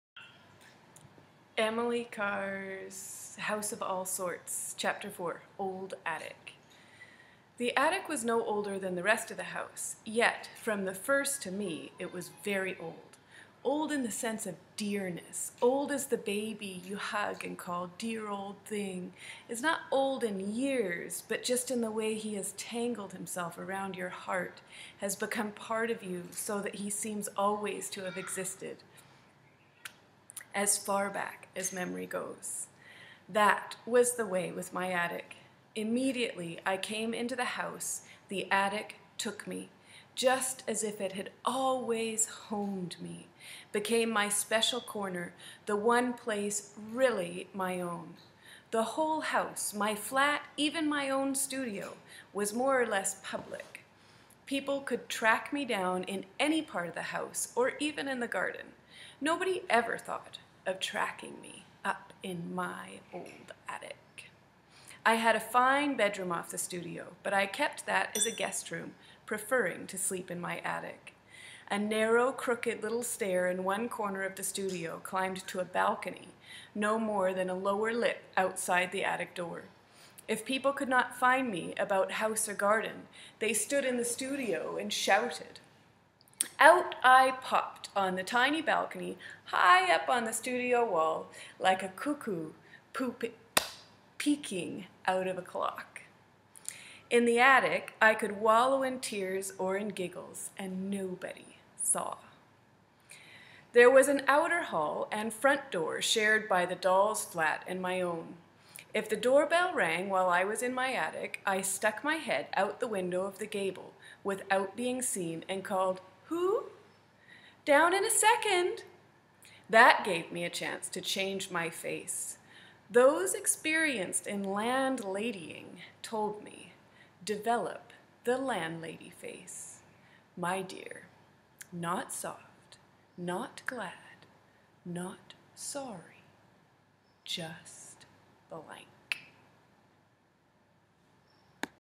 Emily Carr's The House of All Sorts, Chapter 4: Old Attic
A reading of the chapter. I am warmed to find that she, too, needed to spend time being herself with no one watching: "In the attic, I could wallow in tears or in giggles; nobody saw."